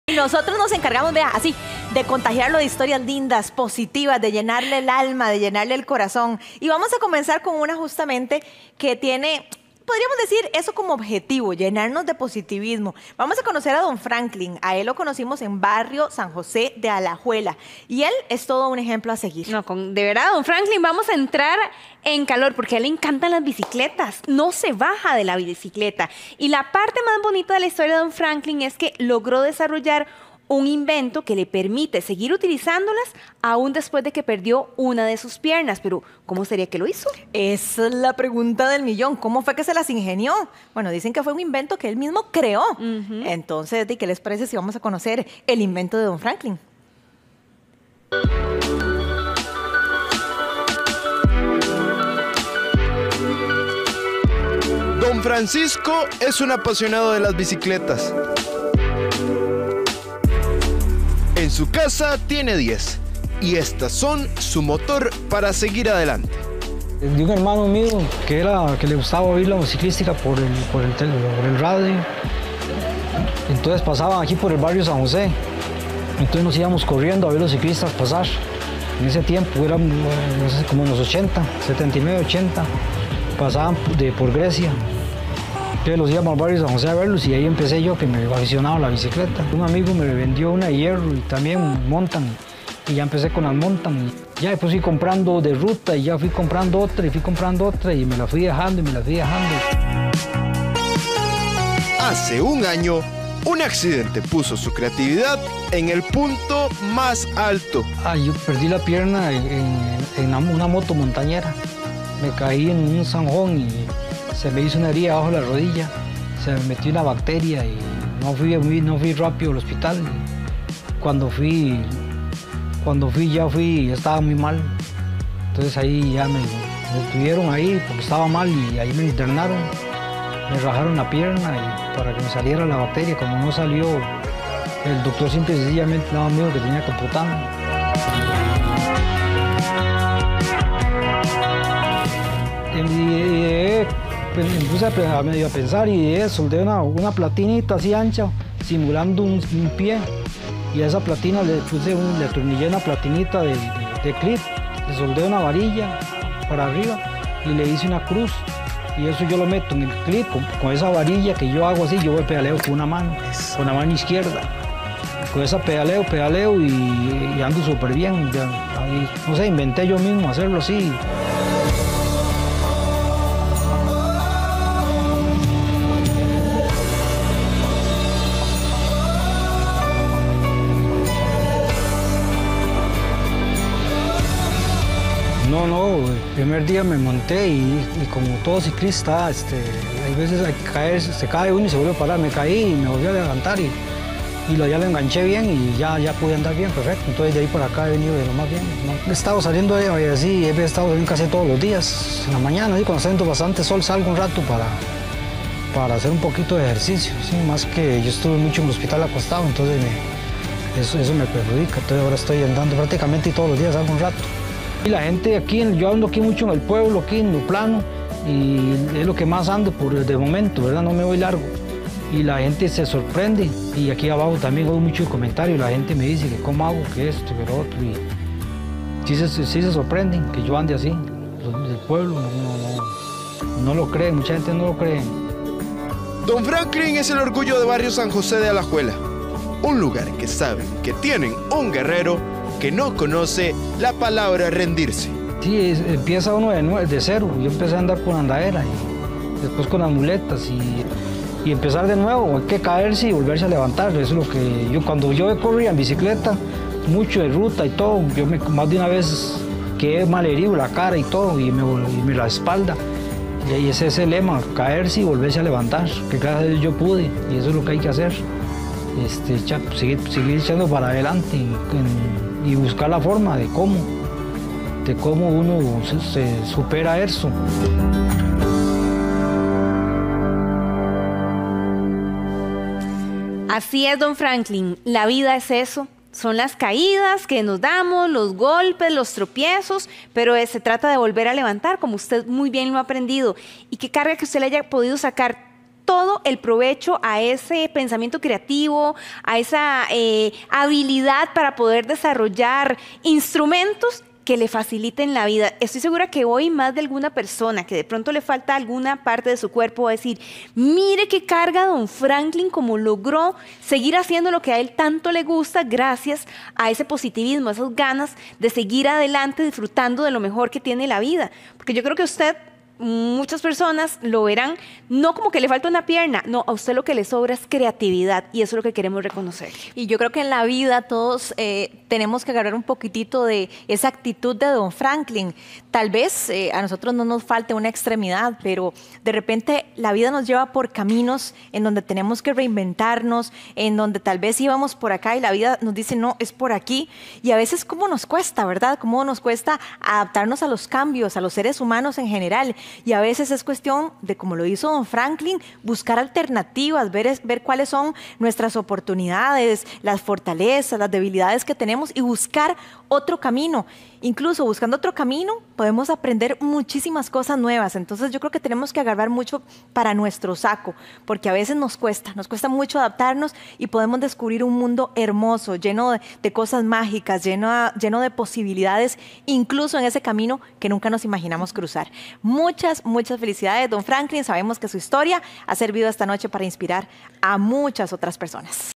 El innovador invento de Don Franklin
Un desafortunado accidente marcó su vida, pero, lejos de rendirse, usó su creatividad para continuar con su pasión: el ciclismo.
Con gran actitud, este hombre afronta la vida, levantándose cuando se cae y nunca dejándose vencer.